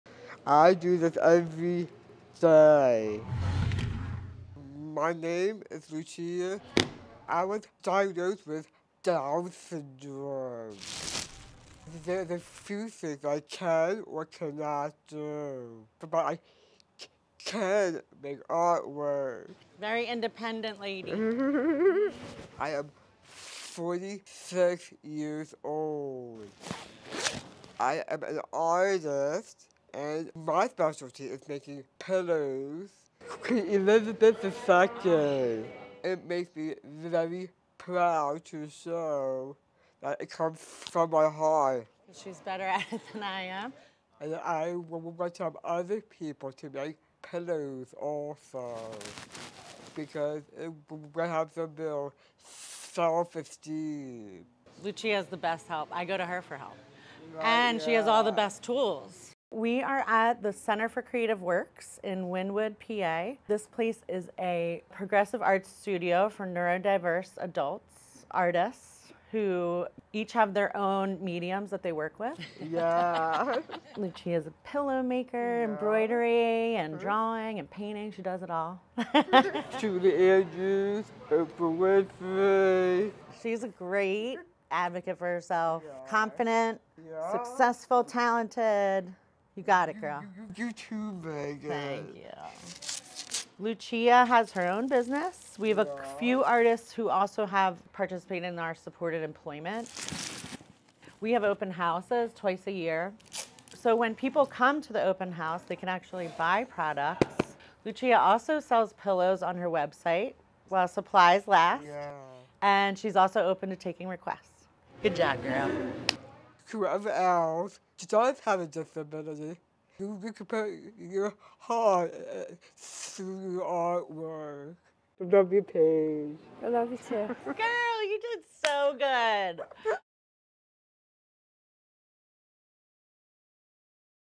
Pa. artist with Down syndrome crafts pillows to share with community
This artist handcrafts colorful pillows and encourages other adults with disabilities to follow their artistic passions. 💙💛